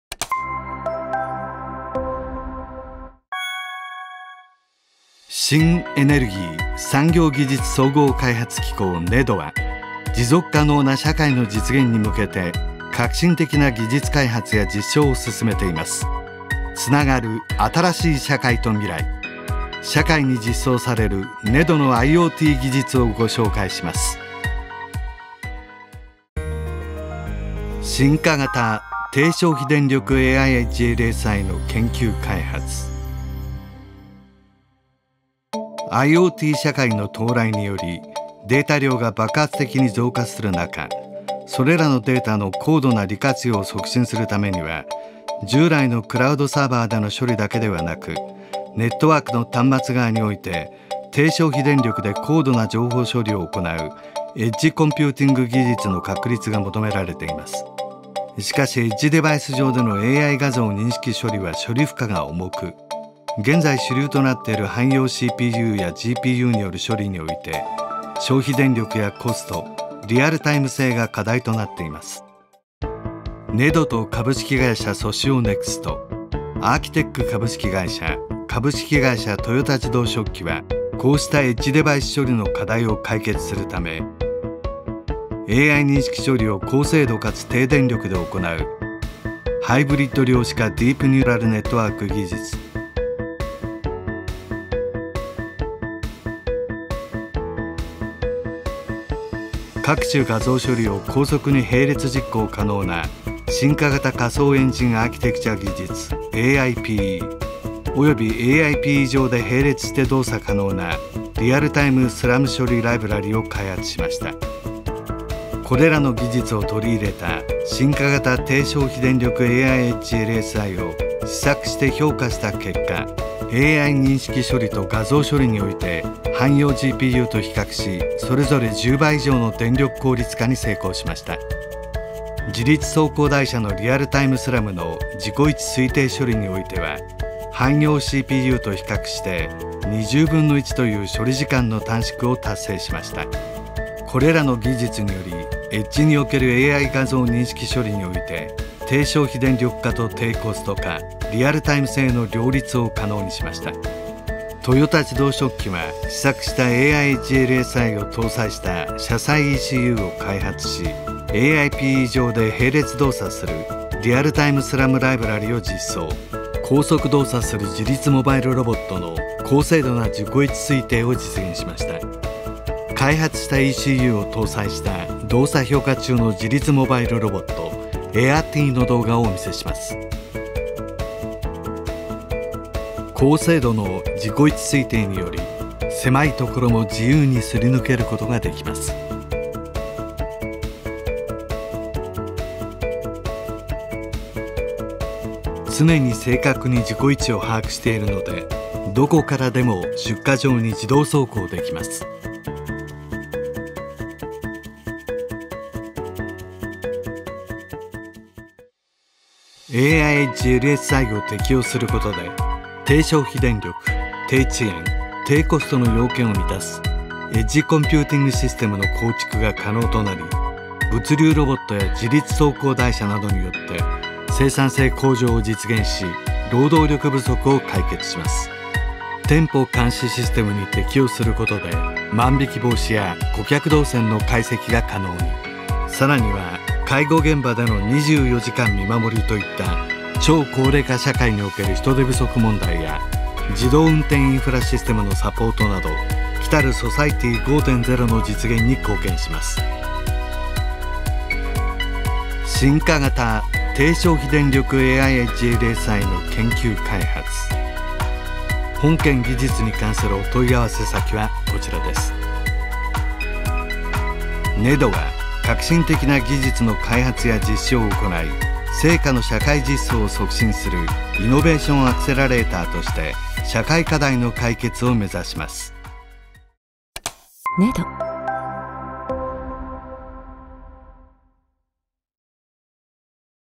【CEATEC2020】未来社会をもっと便利に！進化型・低消費電力AIエッジLSI
【プロジェクト名】高効率・高速処理を可能とするAIチップ・次世代コンピューティングの技術開発

【事業概要】IoT社会の到来によりデータ量が爆発的に増加する中、それらのデータの高度な利活用を促進するためには、従来のクラウドによるデータ処理だけではなく、ネットワークの末端（エッジ）側において低消費電力で高度な情報処理を行う「エッジコンピューティング技術」の確立が求められています。しかし、エッジデバイス上でのAI画像認識処理は処理負荷が重く、現在、主流となっている汎用CPUやGPUによる処理において、消費電力やコスト、リアルタイム性が課題となっています。こうした背景の下、本事業では、人工知能（AI）認識処理、および各種画像処理、リアルタイムSLAM処理技術の研究開発を推進してきました。

【成果】AI認識処理を高精度かつ低消費電力で行うハイブリッド量子化ディープニューラルネットワーク（DNN）技術、各種画像処理を高速に並列実行可能な進化型仮想エンジンアーキテクチャ技術（aIPE）、およびaIPE上で並列して動作可能なリアルタイムSLAM処理ライブラリを開発しました。これらの技術を取り入れた進化型・低消費電力AIエッジLSIを試作して評価した結果、AI認識処理と画像処理において汎用GPUと比較してそれぞれ10倍以上の電力効率化に成功しました。また、自律走行台車のリアルタイムSLAMの自己位置推定処理において、汎用CPUと比較して1／20の処理時間の短縮を達成しました。

【社会実装イメージ】本技術を用いたAIエッジLSIを、物流やマシンビジョン、セキュリティ・見守り、車載センシングシステムなどに適用することにより、低消費電力、低遅延、低コストの要件を満足するエッジコンピューティングシステムの構築が可能となり、急増するデータの高度な利活用促進を加速し、末端（エッジ）側での処理の分散化を実現するAIエッジ技術として、超低消費電力社会の実現に貢献します。